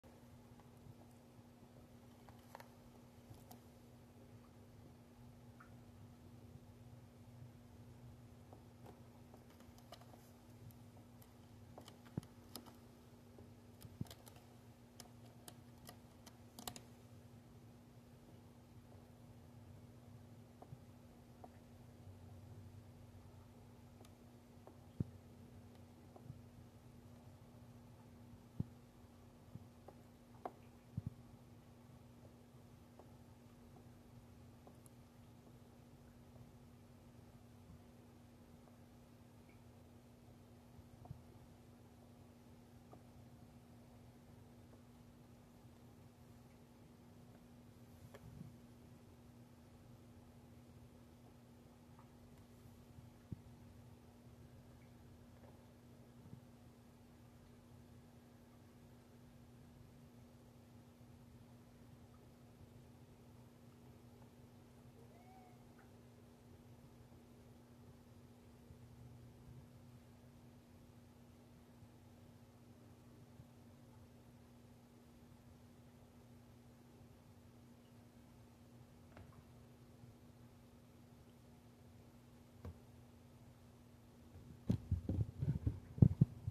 Cat Playing with Wagging Tail
Peaches enjoys playing with a wagging tail.